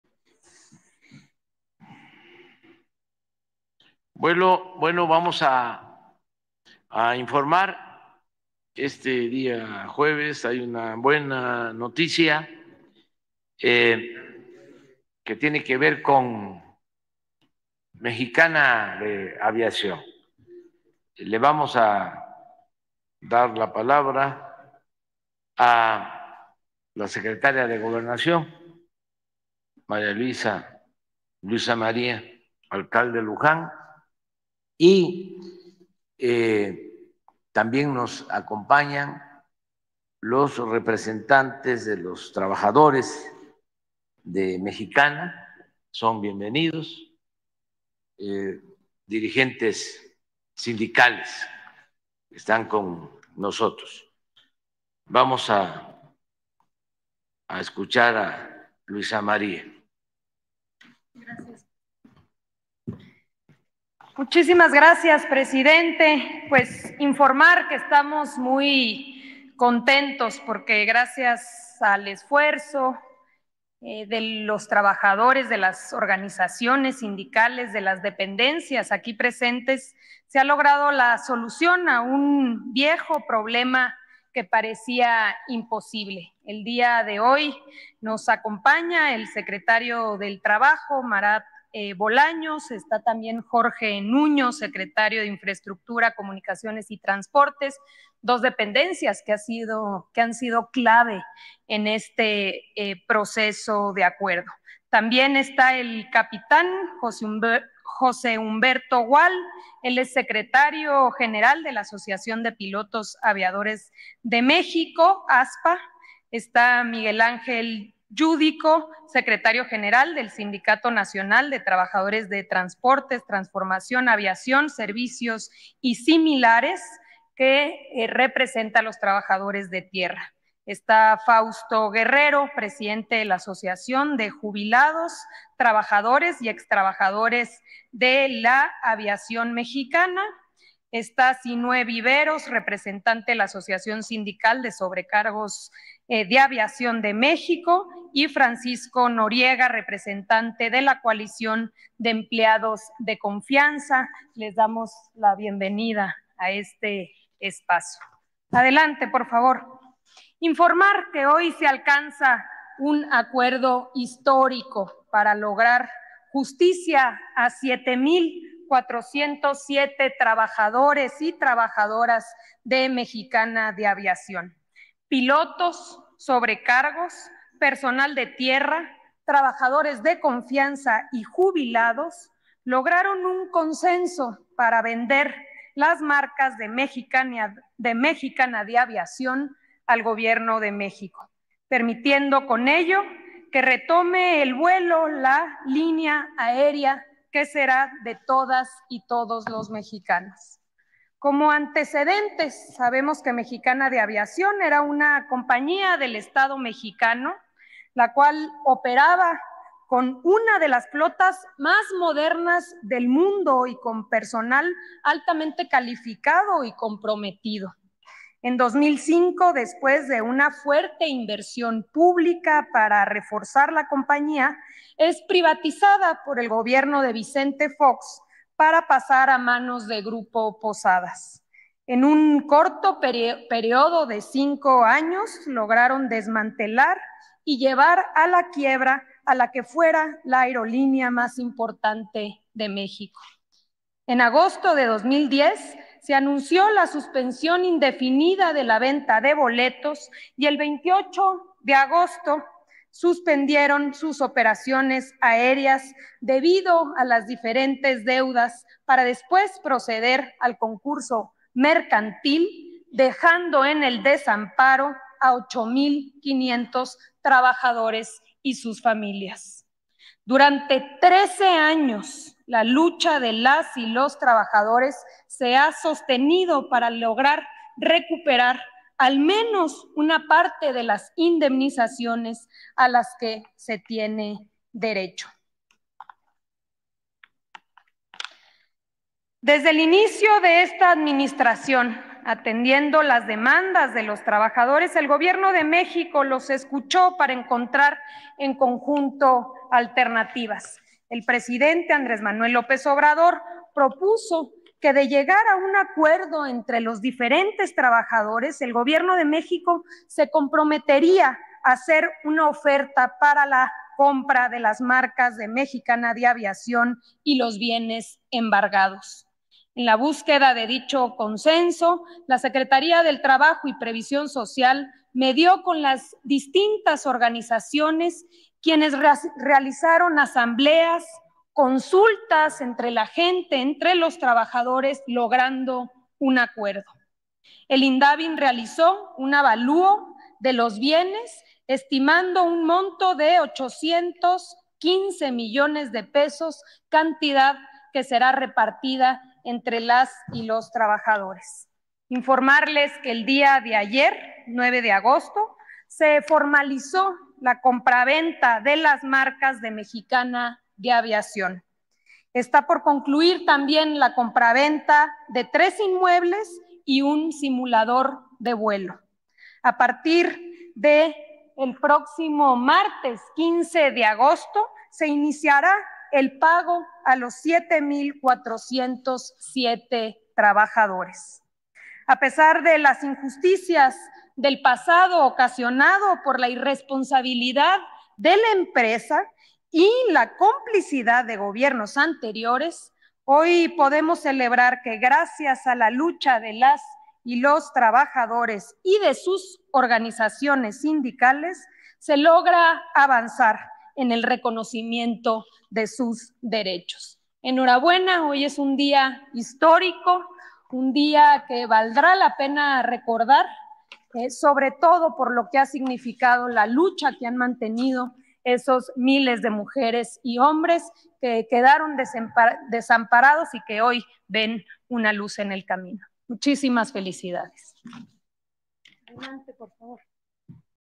AMLO CELEBRA ACUERDO PARA QUE MEXICANA DE AVIACIÓN RETOME EL VUELO
El presidente López Obrador comenzó su mañanera asegurando que hay una muy buena noticia que tiene que ver con Mexicana de Aviación. Para esto, le dio la palabra a la secretaria de Gobernación Luisa María Alcalde quien planteó que se logró la solución a un viejo problema que parecía imposible. Dijo que hoy se alcanzó un acuerdo para darle justicia a más de siete mil trabajadores de Mexicana de Aviación al vender las marcas al Gobierno de México, permitiendo con eso que retome el vuelo esa aerolínea.